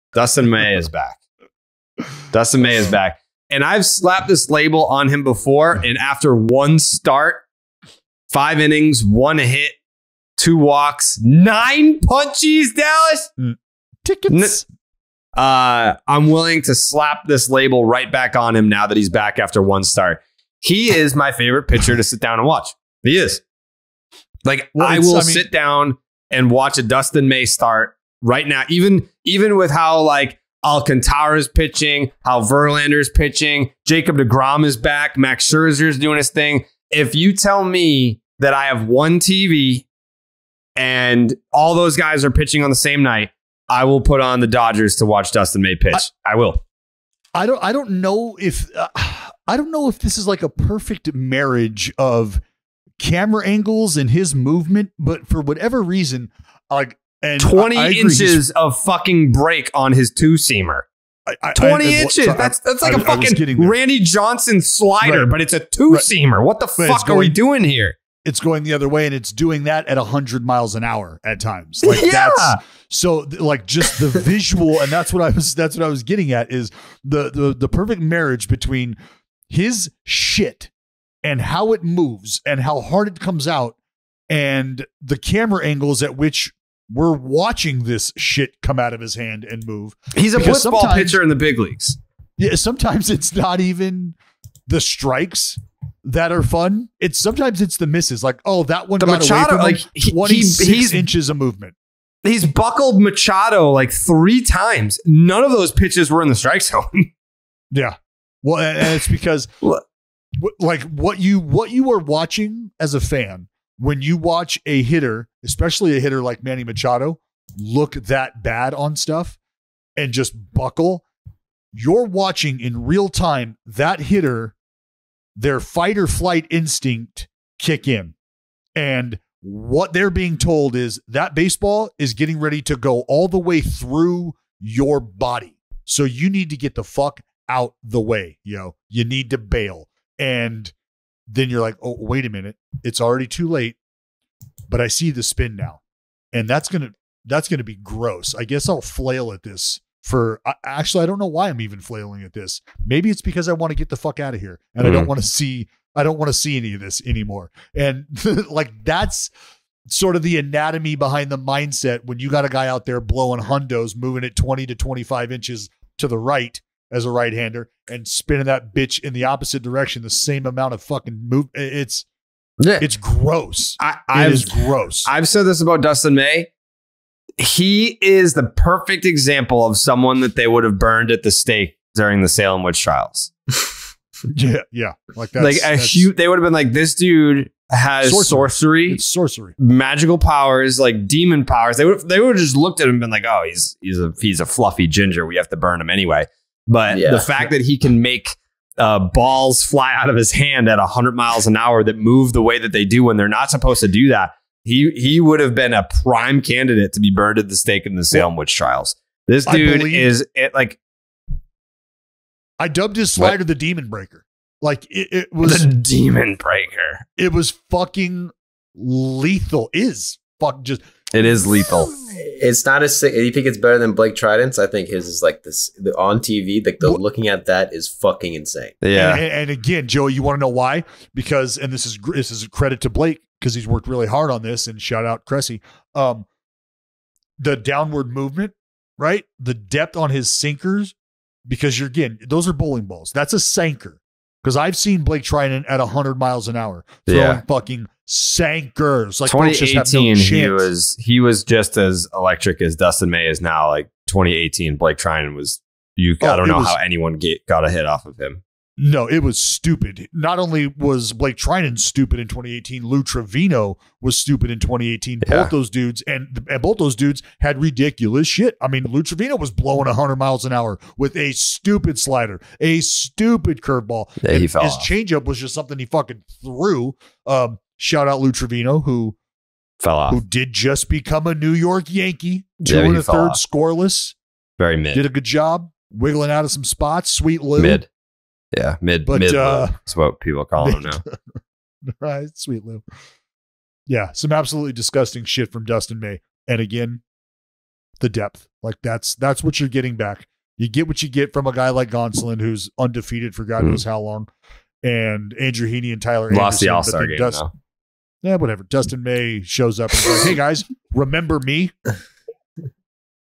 The most entertaining pitcher in baseball is back
Jared, Dallas, and @BaseballDoesntExist talk about Dustin May's return from Tommy John surgery and how excited Jared is to have the Dodgers pitcher return to the mound. Is he the most exciting young pitcher in baseball?